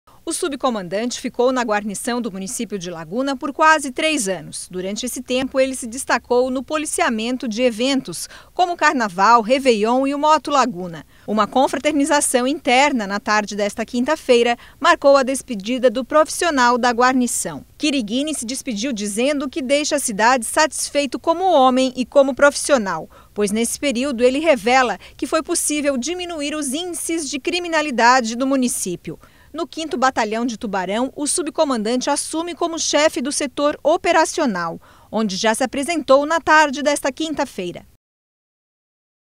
Major Chierighini é transferido de Laguna para Tubarão
O Major Dante da Costa Chierighini foi transferido da Guarnição Especial de Polícia Militar de Laguna para o quinto Batalhão de Polícia Militar de Tubarão